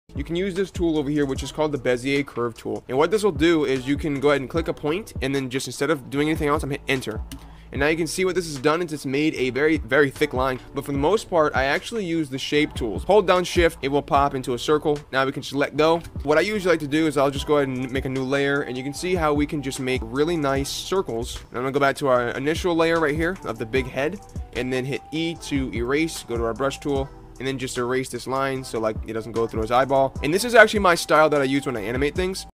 How to Draw With A Mouse
How to draw with a mouse in ANY drawing application (I'm using Krita in the video) without a drawing tablet!